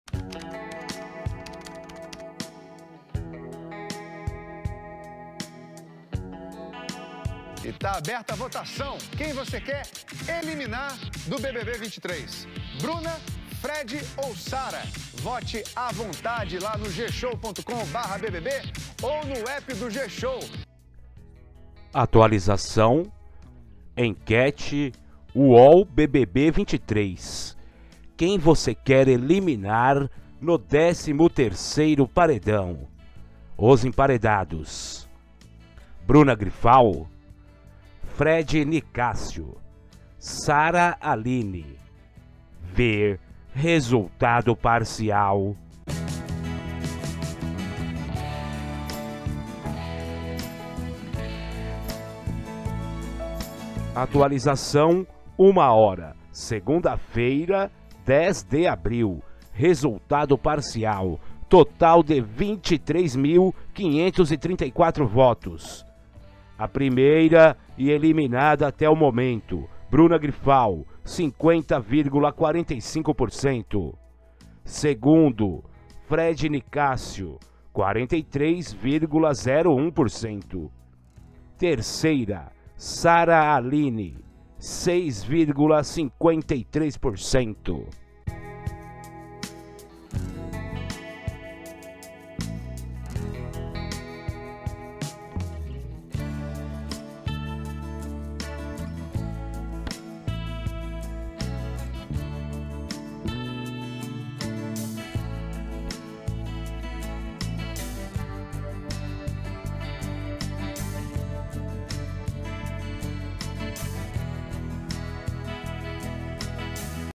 ENQUETE UOL BBB 23 atualizado 1h dia 10 '13º paredão' virada quem vai ser eliminado
Resultado parcial
Total de 23534 votos
50,45%Divulgação/Globo
Bruna Griphao
imagem: Divulgação/Globo
43,01%Divulgação/Globo
Fred Nicácio
imagem: Divulgação/Globo
6,53%Divulgação/Globo
Sarah Aline